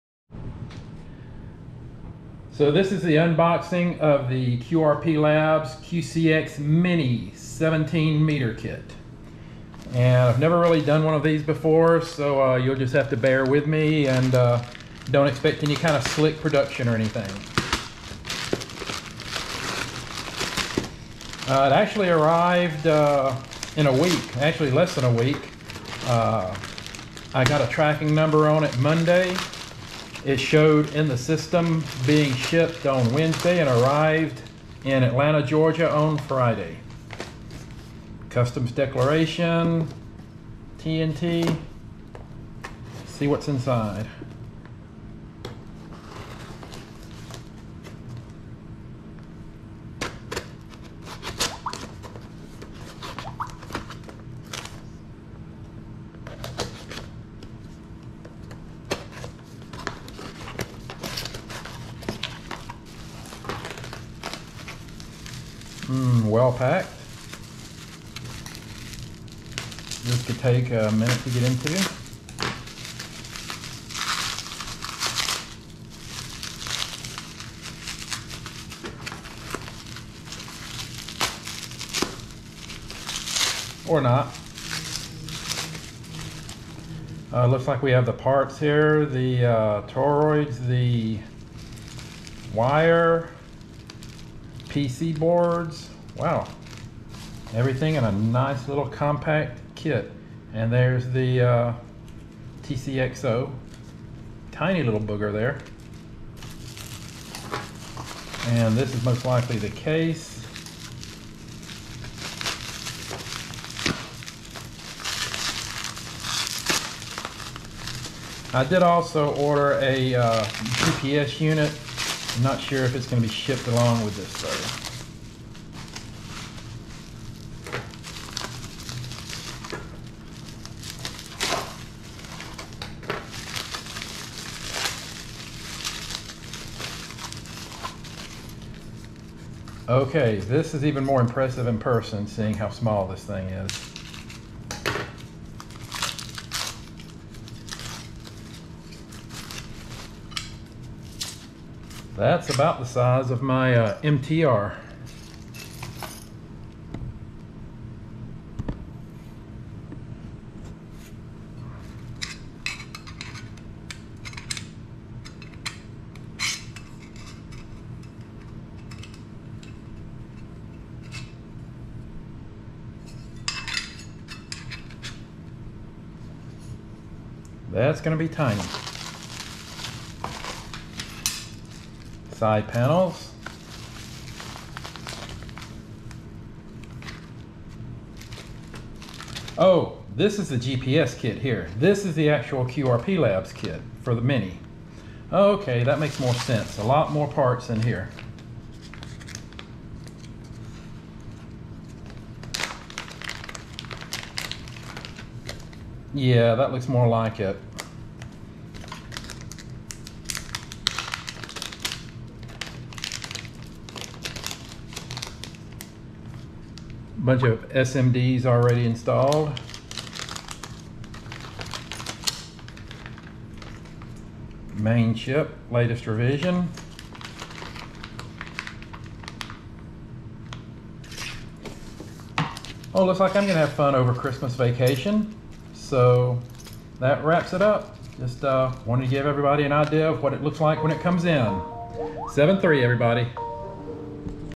QCX Mini and GPS unboxing
Just a quick unboxing video of the QCX Mini and GPS module.